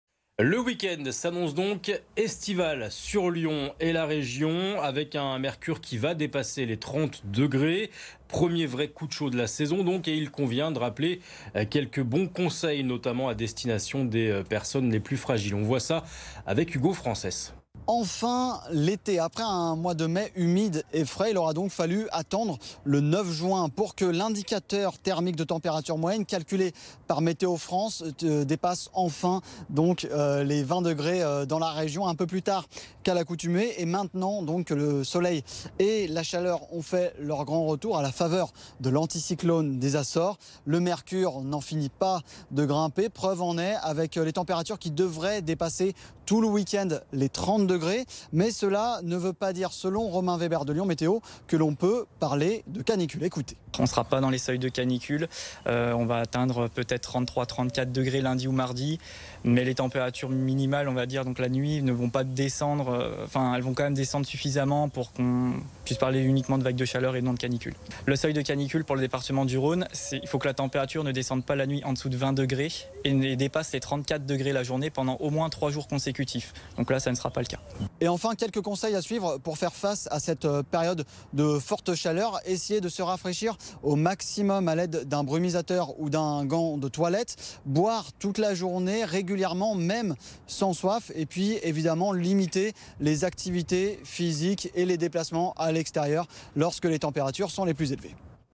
Le week-end sera chaud sur Lyon !
Soleil et chaleur au programme tout ce week-end à Lyon et dans la région. Il convient de rappeler quelques consignes, notamment pour les personnes les plus fragiles